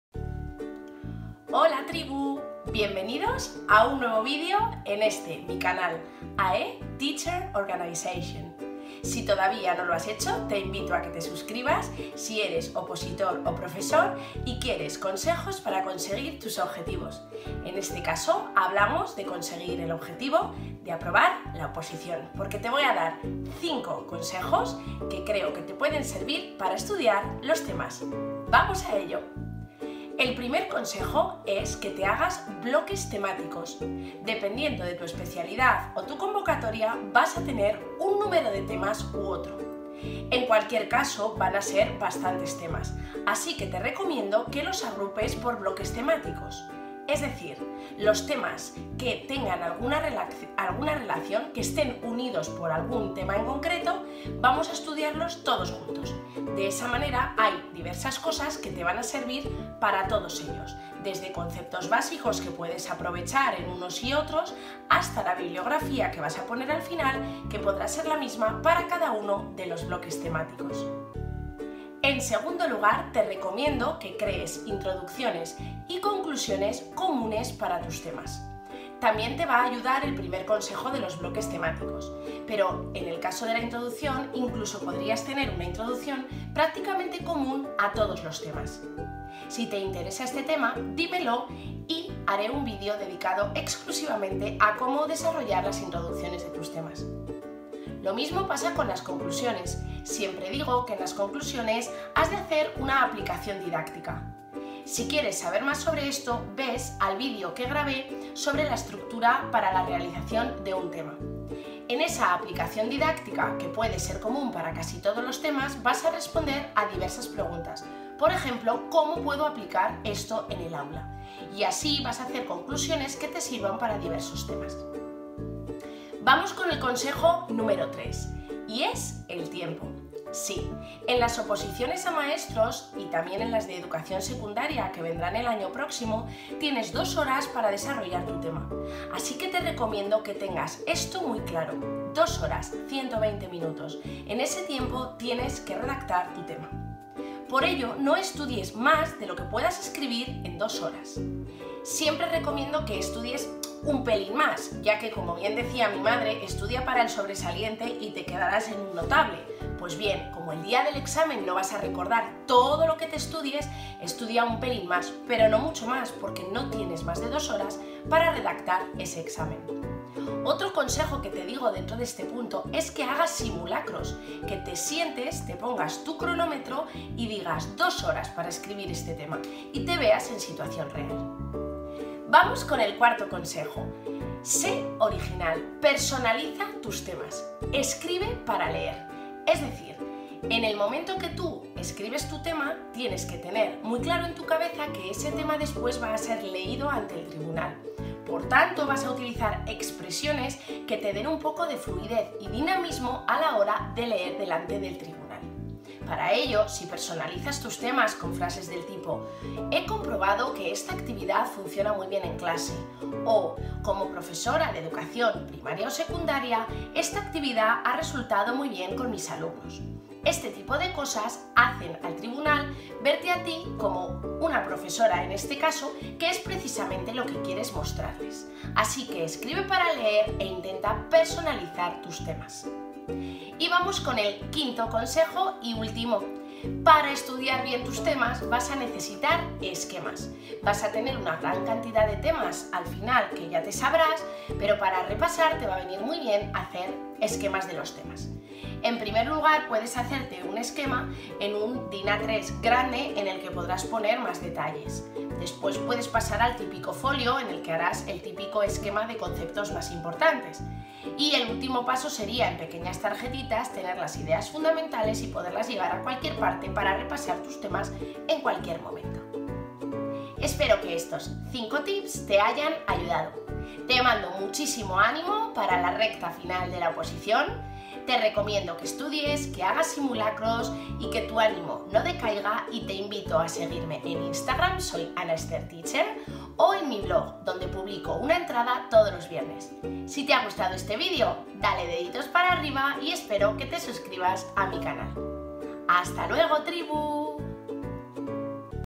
5 CONSEJOS PARA ESTUDIAR LOS TEMAS DE LA OPOSICIÓN
No te pierdas nada! Consejos de organización para conseguir tus objetivos. Si eres profe u opositor, ésta es tu TRIBU!